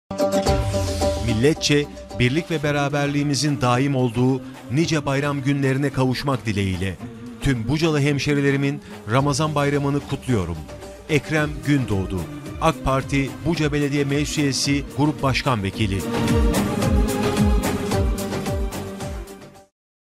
AKPARTİ BUCA BELEDİYE MECLİS ÜYESİ GRUP BAŞKAN VEKİLİ SAYIN EKREM GÜNDOĞDU'NUN BAYRAM MESAJI!
AKPARTİ BUCA BELEDİYE MECLİS ÜYESİ GRUP BAŞKAN VEKİLİ  SAYIN EKREM GÜNDOĞDU'NUN BUCA HALKINA BAYRAM MESAJI!